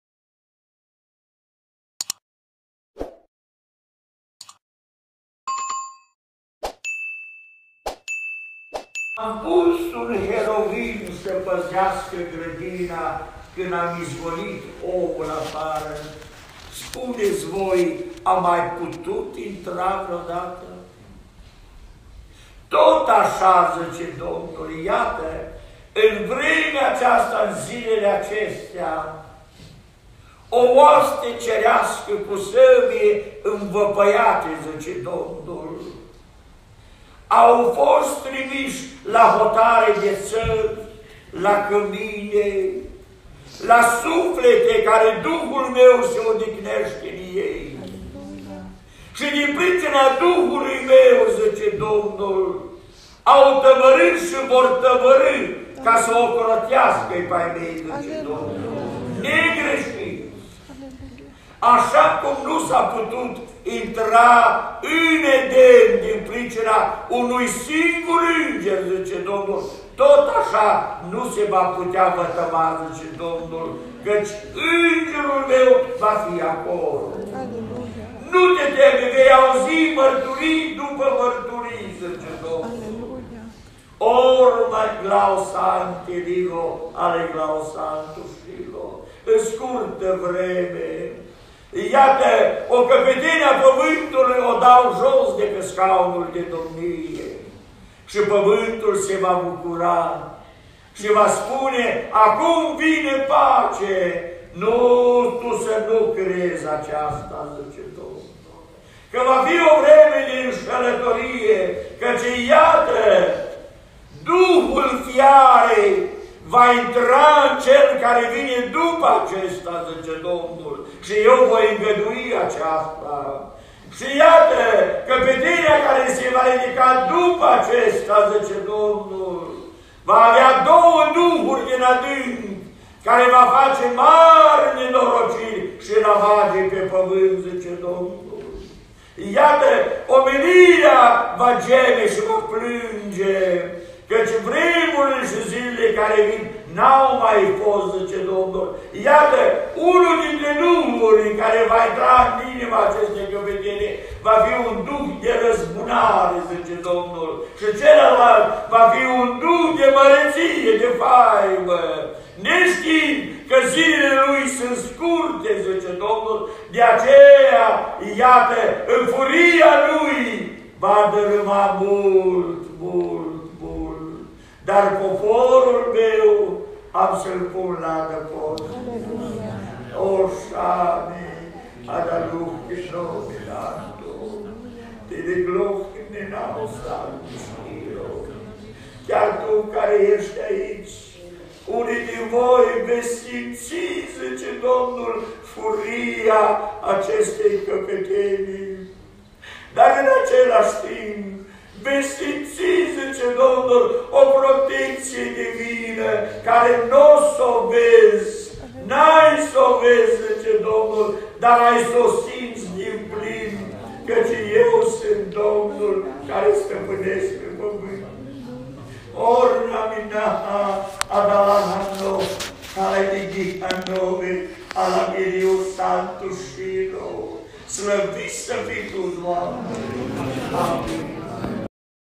Prorocie - Oaste cerească, dau jos o căpetenie a pământului de la domnie, va veni duhul fiarei
fr Aurel Balici (Chicago)